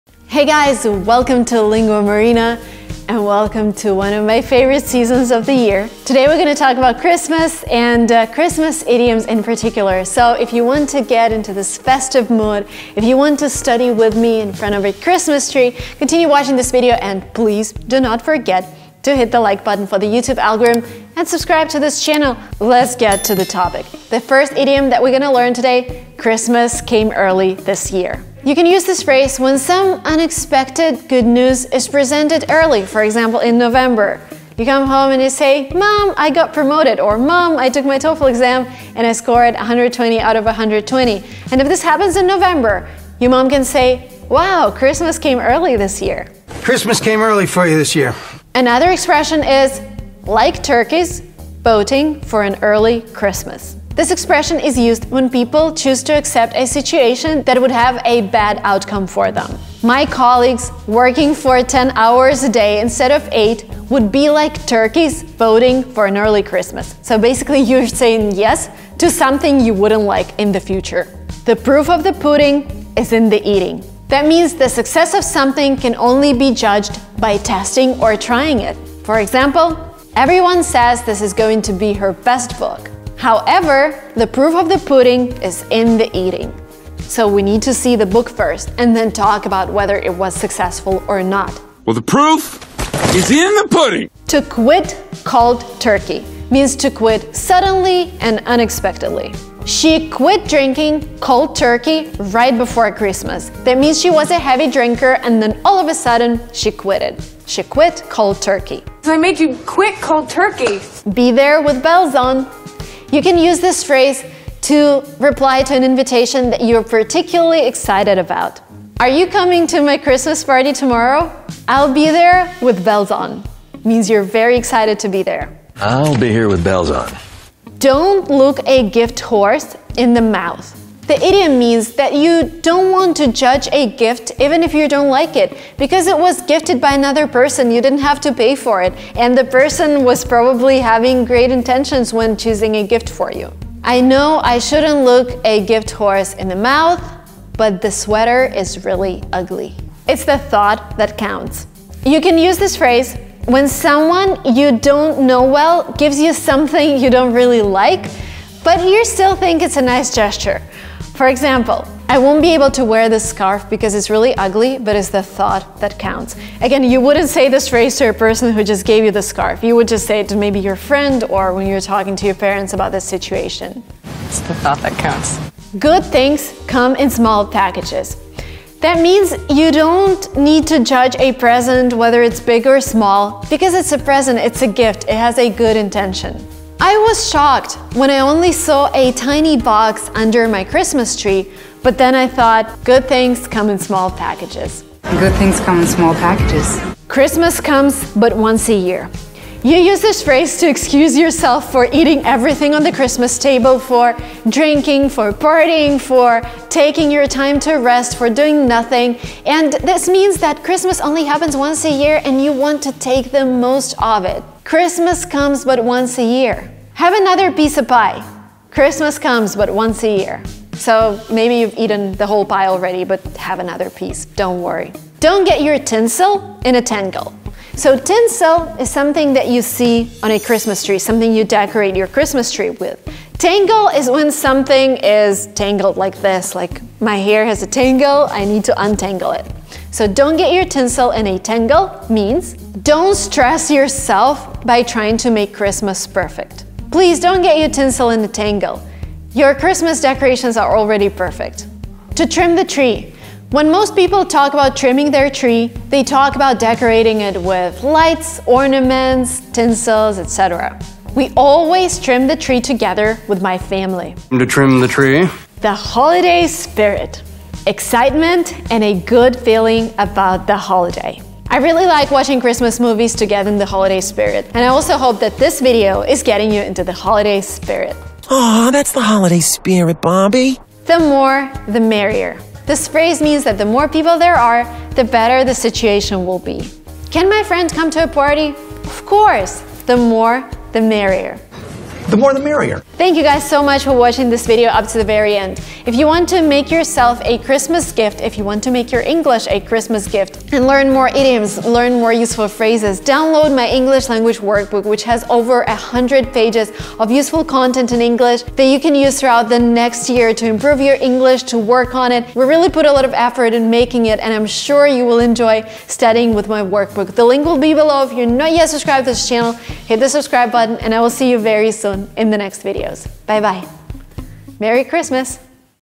13 Must-Know Christmas Idioms and Phrases
Let's learn 13 English idioms and phrases that are related to Christmas and that will help you sound like a native speaker.

0:00 Christmas idioms and phrases:
0:24 Christmas came early this year
0:48 Like turkeys voting for an early Christmas
1:11 The proof of the pudding is in the eating
1:34 To quit cold turkey
1:51 Be there with bells on
2:08 Don’t look a gift horse in the mouth
2:30 It’s the thought that counts
2:55 Good things come in small packages
3:17 Сhristmas comes but once a year
3:45 Don’t get your tinsel in a tangle
4:12 To trim the tree
4:25 The holiday spirit
4:41 The more the merrier